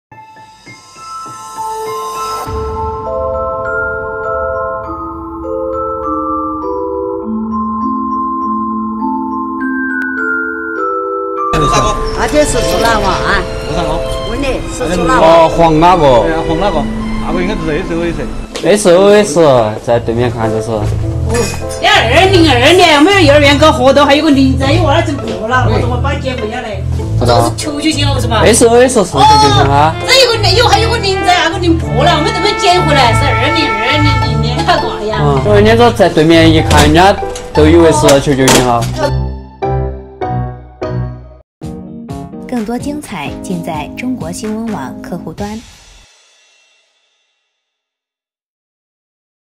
2020字样气球少了个0 邻居误认为SOS报警求助
近日，贵州开阳民警接到报警称：居民楼有一家窗户贴着SOS求救信号。在民警挨家挨户寻找到了“求救”地点后，结果却让人哭笑不得。原来，房子主人将“2020”字样的气球贴在客厅阳台的窗户上，其中有一个0因为漏气就没贴上去，没想到从外面看就是“SOS”求救信号。